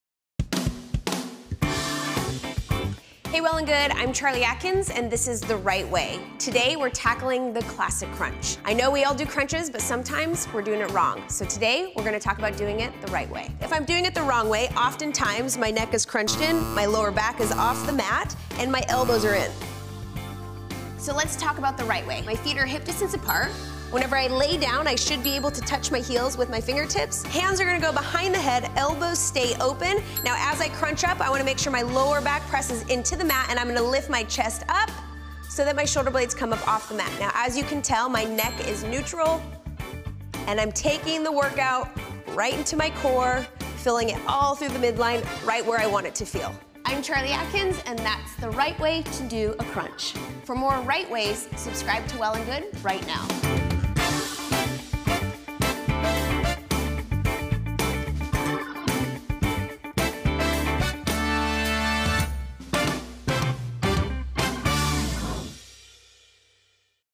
How To Do Crunches | The Right Way | Well+Good
Crunches are one of the best ways to get those six pack abs. But do you know the right way to do a crunch? Trainer Charlee Atkins of Le Sweat and Le Stretch shows us how to do crunches the right way. No injuries here! Up your core exercises by following her simple tips.